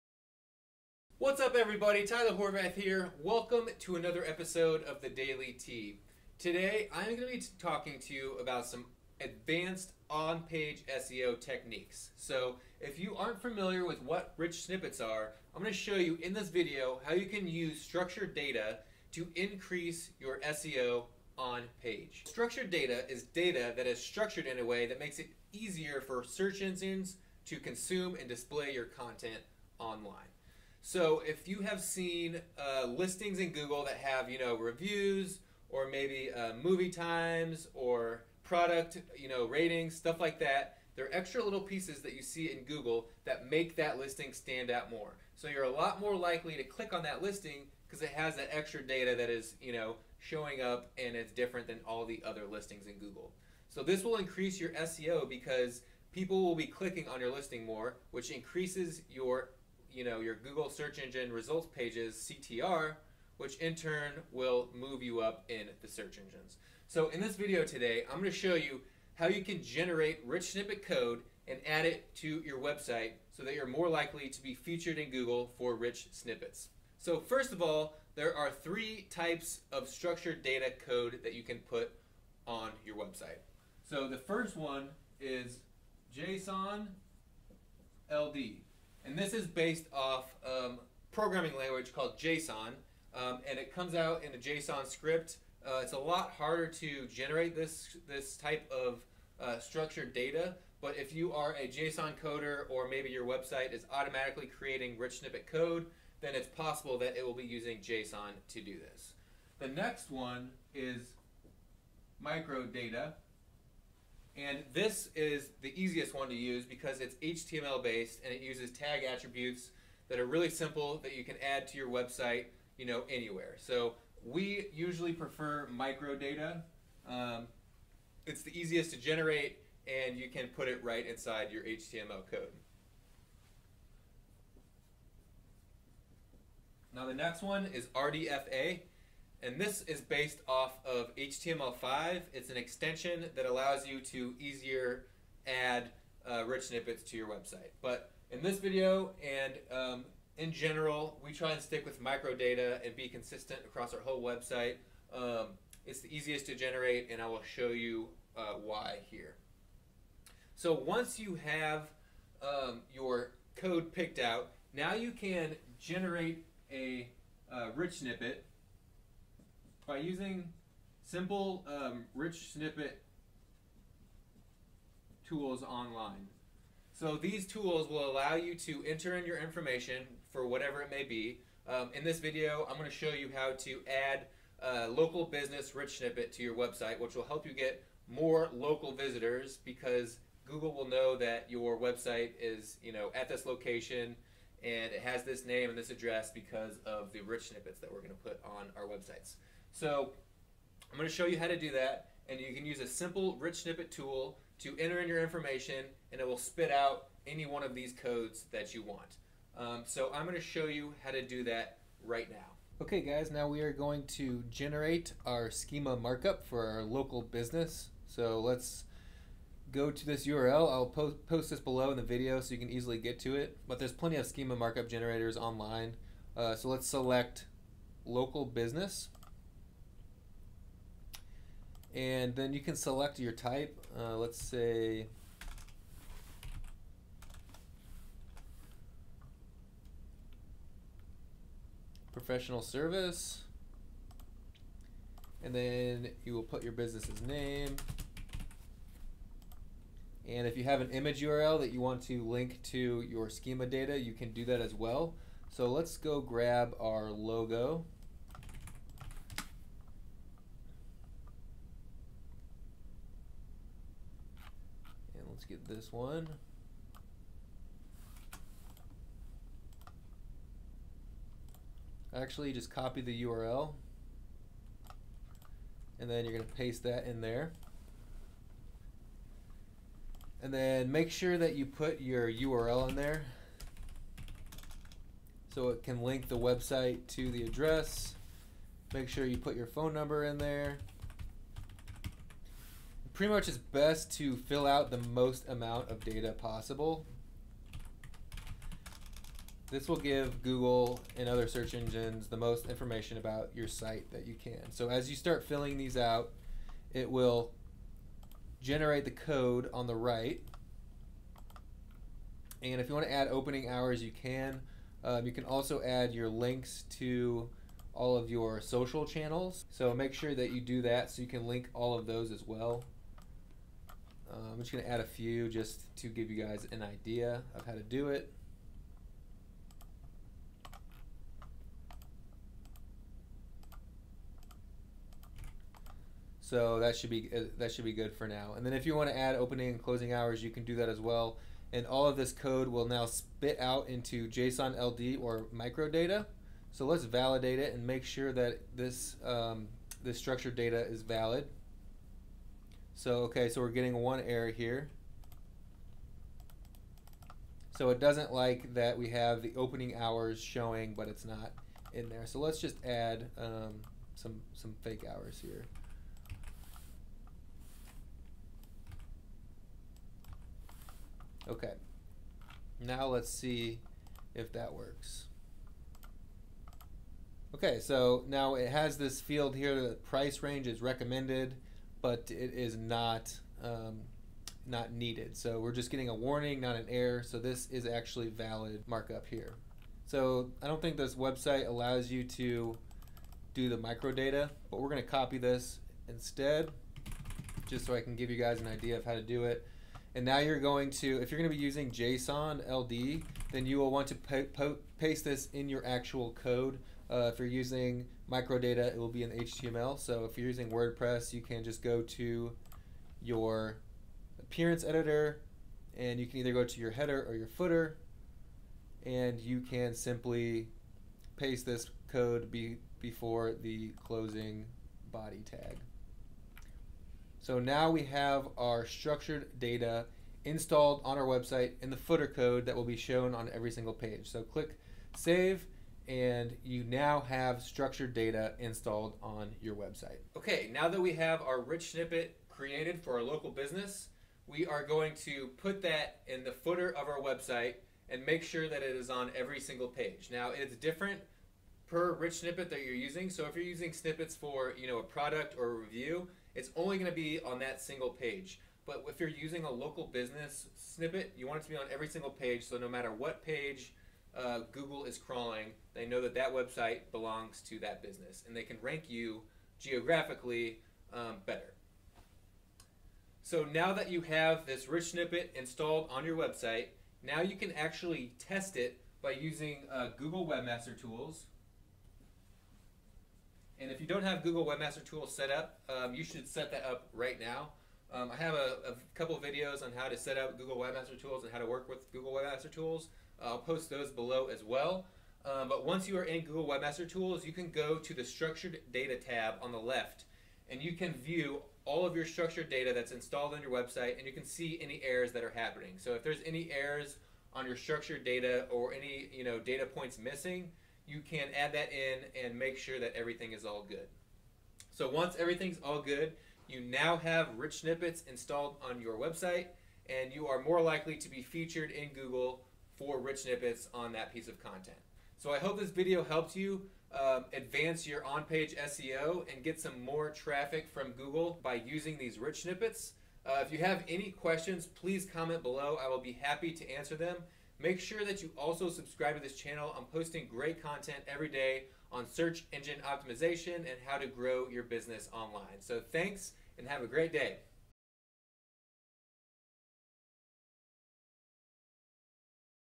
Using Rich Snippets to Increase SEO | Tyler Horvath
Structured data is a somewhat new way to optimize your website for on-page SEO.  It’s often looked over by webmasters because it’s new.. and also because it seems very confusing and difficult.

Schema markup is a code snippet that you add in your HTML web pages to help search engines consume your data and display this data to searchers.

Schema.org is a collaboration project between some of the biggest search engines (as well as other websites) to create a standardized structured markup. Schema.org contains documentation of every schema and available variables for each schema.

Some examples of schemas are:

Events
Organization
Local Business
Place
Product
Review
Book
Movie
Recipe

Types of Schema Syntax
JSON-LD: Javascript notation that is included in the head section inside a script tag.

Microdata: HTML tag attributes to name the properties you want to define as structured data.

RDFa: HTML5 extension that supports linked data with HTML tag attributes that correspond to user-visible content you want to define for search engines.